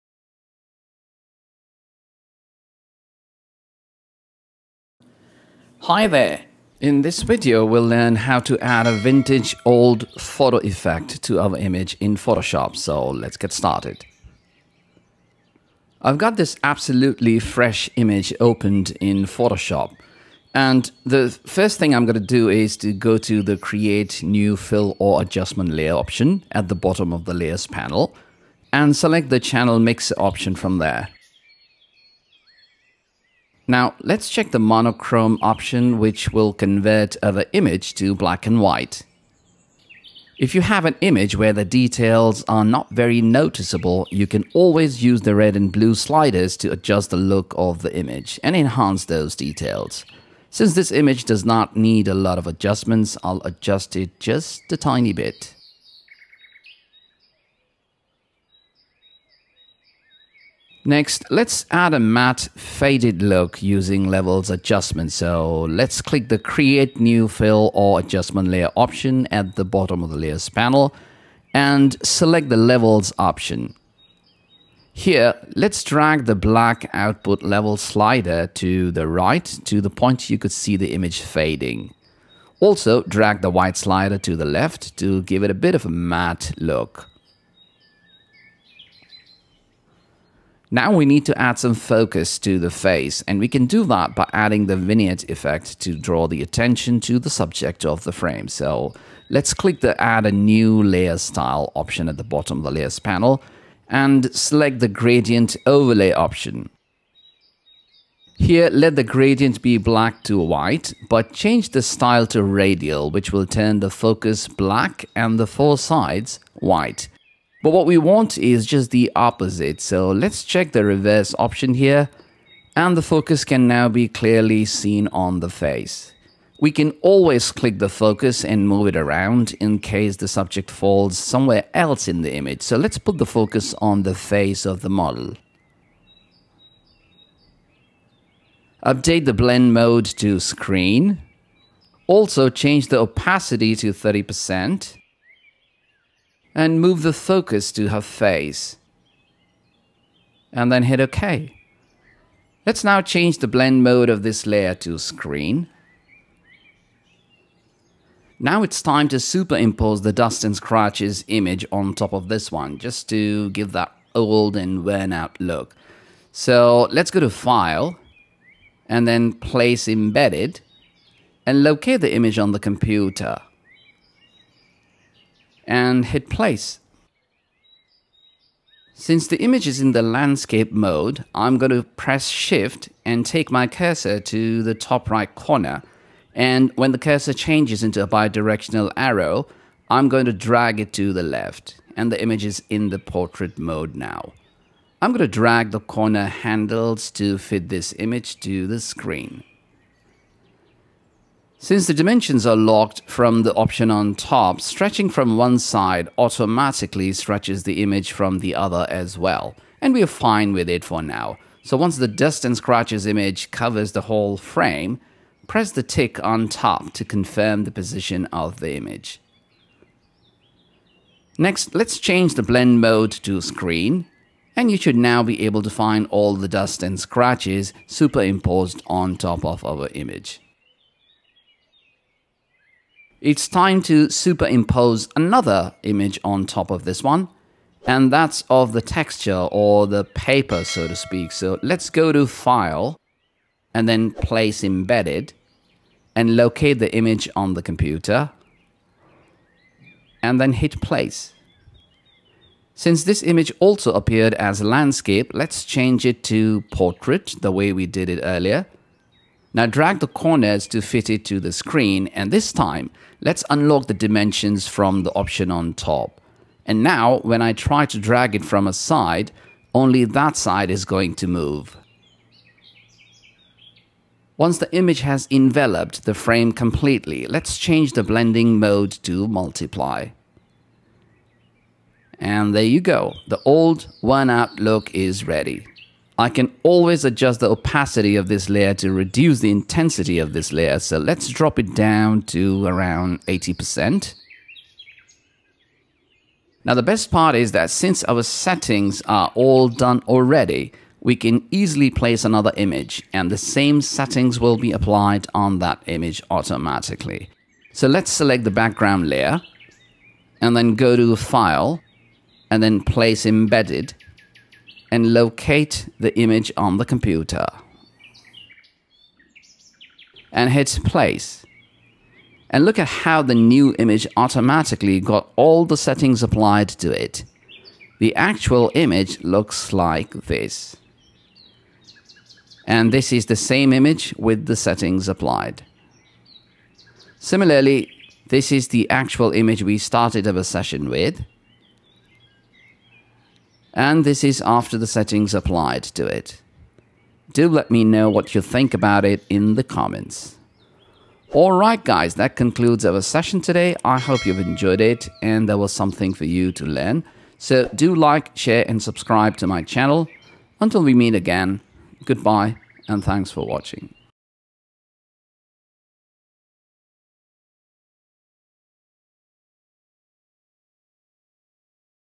Add a cool Old Vintage effect to your image in Photoshop
This video illustrates how to add a cool Old Vintage effect to your image in Photoshop.